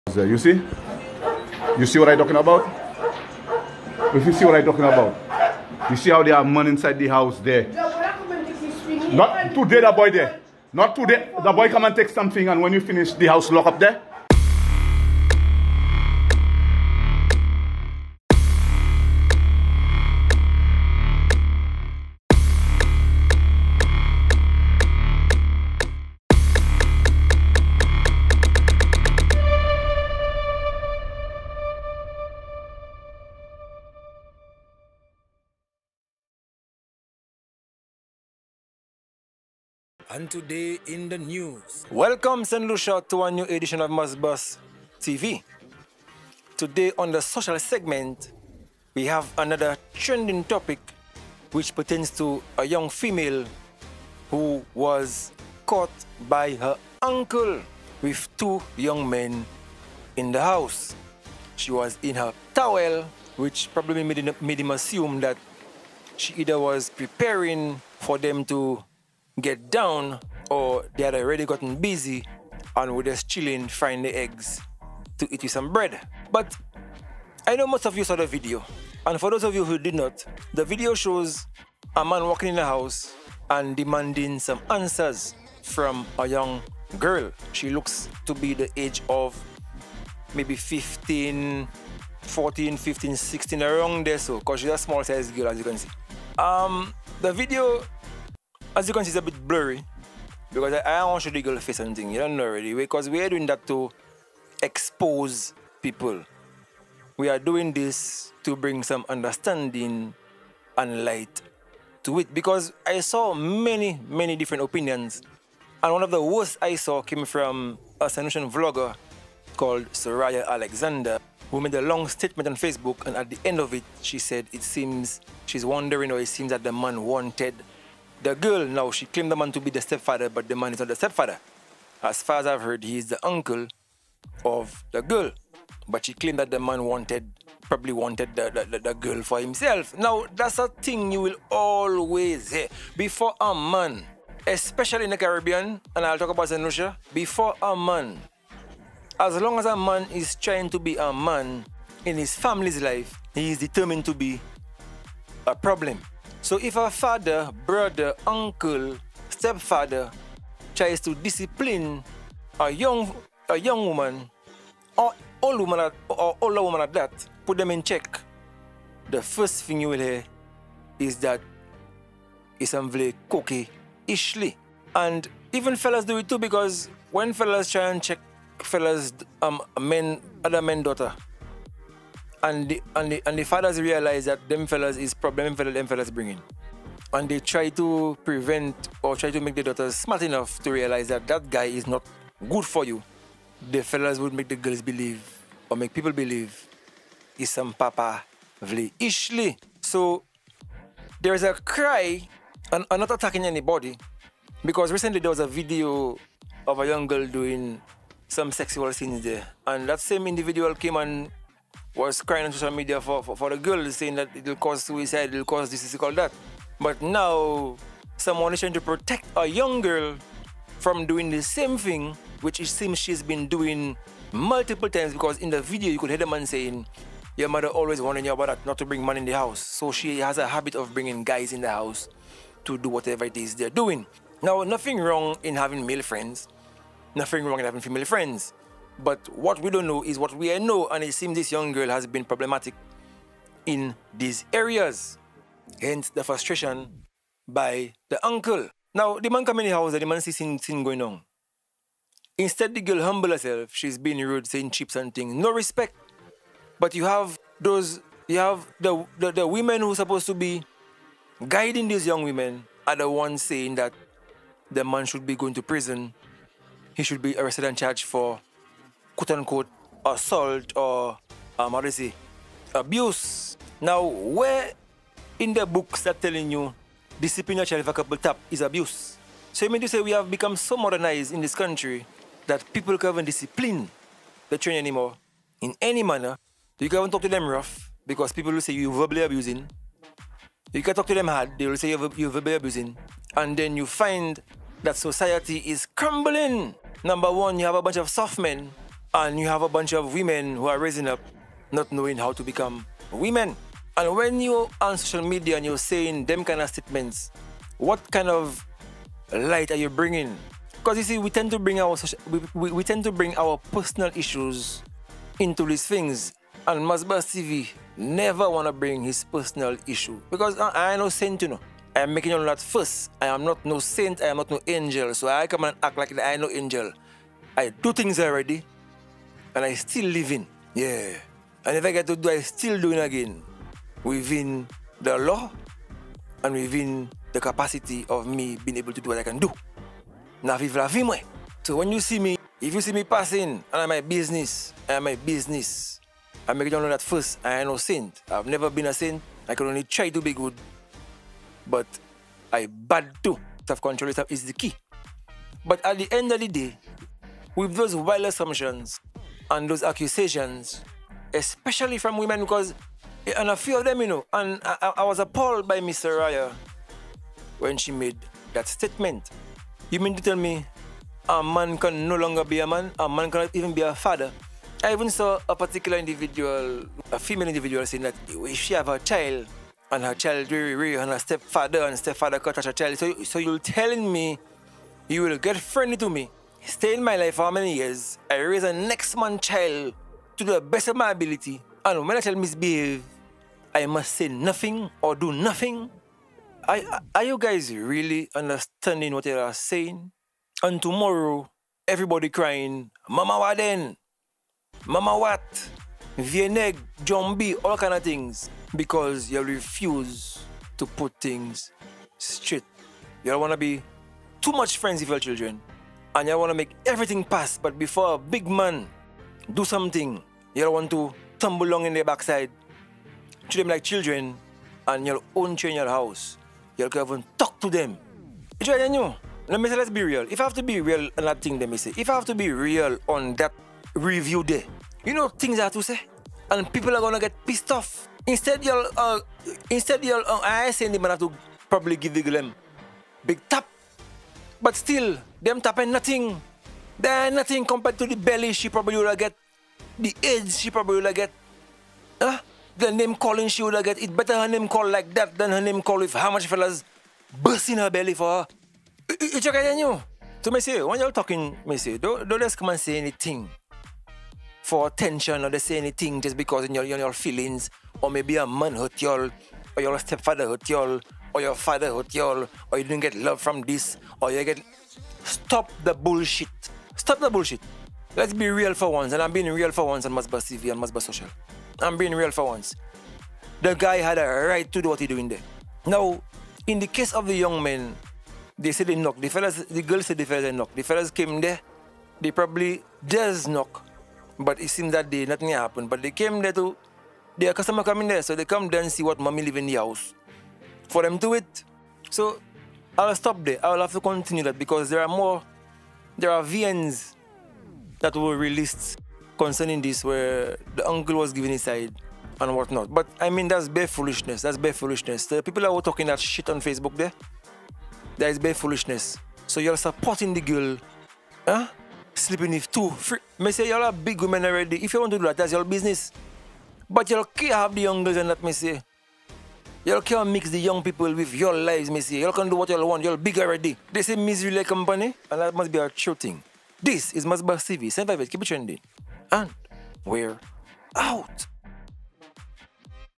Don't BRING MAN IN THE house. VIRAL vid.
Yet another viral video , THIS TIME  is with a young girl  being disobedient , by refusing to listen to the warnings . Living like this is Dangerous for young girls , and boys , and PARENTS , need to step up to help better the youth .